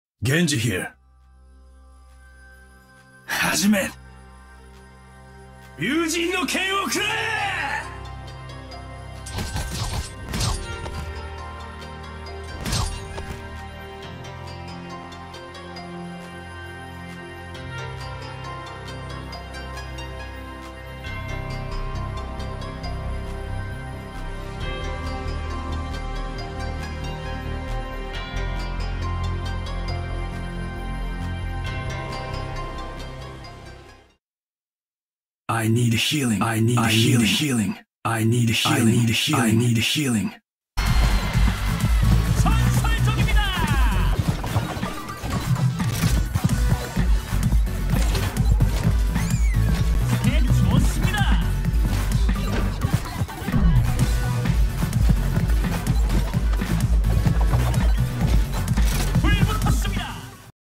Young Genji cosplay from Overwatch
This is my 100% homemade young Genji cosplay!